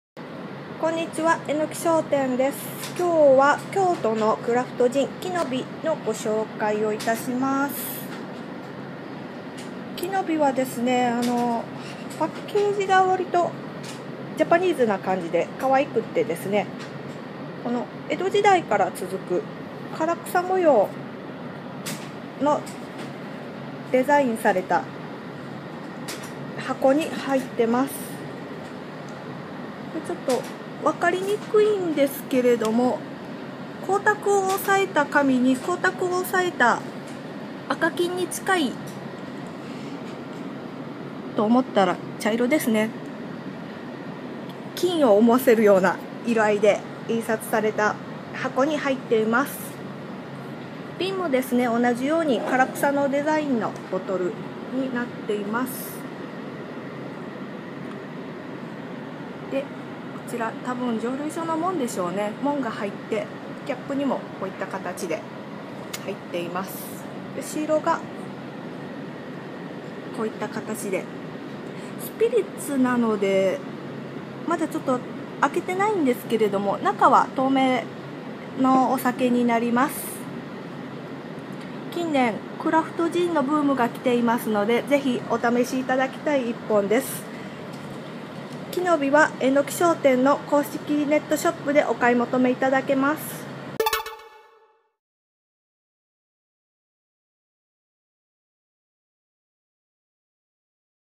お酒通販 季の美(きのび) 箱に伝統文様を使った日本風色の強いジン（GIN）
ご紹介のお酒は榎商店ネットショップで販売中です。